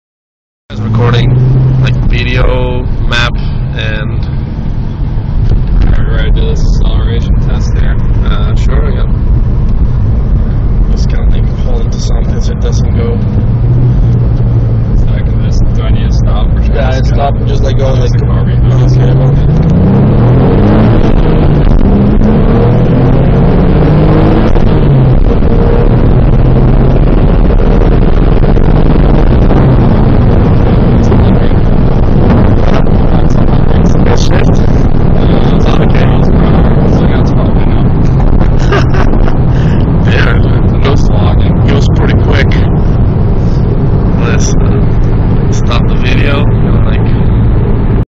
Porsche 911 Turbo run. Torque for Android App.
Quick test run video of the Track Recorder plugin for Torque for Android app on a Porsche 911 Turbo.
For some reason the map didn't show :(